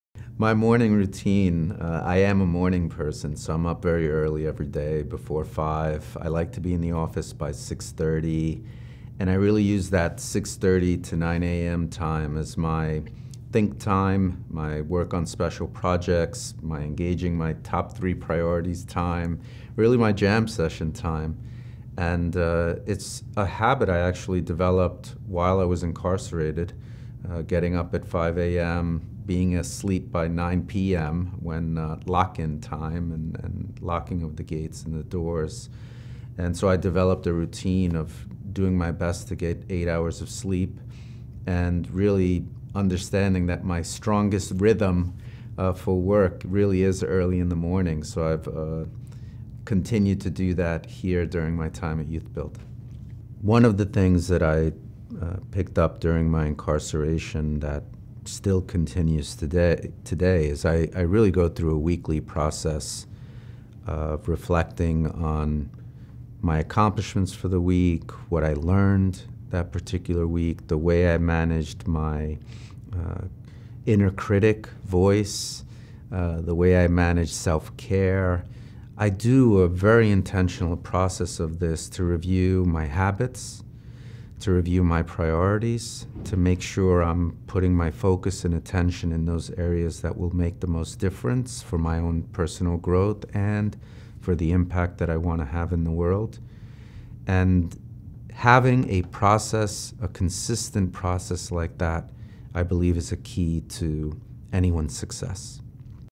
Morning Routines & Habits - John Valverde
John shares with us his morning routine and his weekly self-reflection process which he picked up while incarcerated.